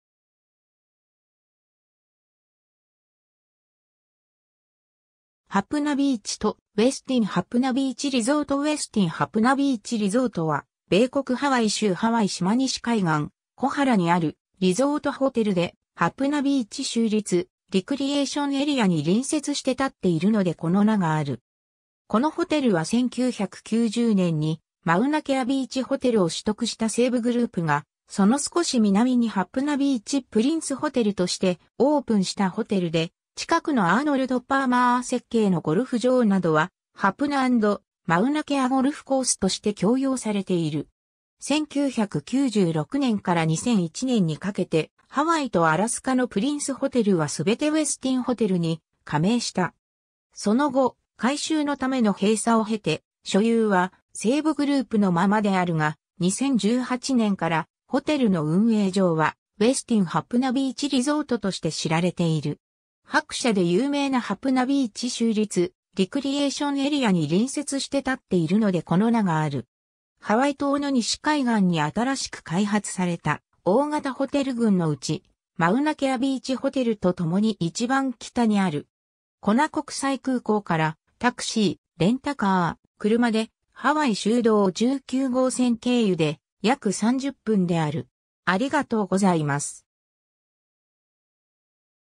ウェスティン・ハプナビーチ・リゾート
スターウッド・ホテル&リゾートのホテル
ハプナビーチとウェスティン・ハプナビーチ・リゾート（後方） ウェスティン・ハプナビーチ・リゾート（英語: Westin Hapuna Beach Resort）は、米国ハワイ州ハワイ島西海岸、コハラにあるリゾートホテル（Resort hotel）で、ハプナビーチ州立リクリエーションエリアに隣接して立っているのでこの名がある。
 このホテルは1990年にマウナケア・ビーチ・ホテルを取得した西武グループが、そのすこし南にハプナビーチ・プリンスホテルとしてオープンしたホテルで、近くのアーノルド・パーマー設計のゴルフ場などは「ハプナ＆マウナケアゴルフコース」として共用されている。
1996年から2001年にかけて、ハワイとアラスカのプリンスホテルはすべてウェスティンホテルに加盟した。
その後、改修のための閉鎖を経て、所有は西武グループのままであるが、2018年からホテルの運営上はウェスティン・ハプナビーチ・リゾートとして知られている。
 白砂で有名なハプナビーチ州立リクリエーションエリア（Hapuna Beach State Recreation Area）に隣接して立っているのでこの名がある。
ハワイ島の西海岸に新しく開発された大型ホテル群（南から北へ、コナ地区：ケアウホウのシェラトン、カイルア・コナのキングカメハメハ・コナビーチ・ホテル、カウプレフのフォーシーズンズホテル、コハラ地区：ワイコロアビーチのヒルトンとマリオット、マウナラニ・リゾートのフェアモント・オーキッド・ホテル、ウェスティン・ハプナビーチ・リゾート／ハプナビーチ・プリンス、カウナオア湾に面したマウナケア・ビーチ・ホテル）のうち、マウナケア・ビーチ・ホテルと共に一番北にある。
 コナ国際空港から、タクシー／レンタカー／車でハワイ州道19号線経由で、約30分である・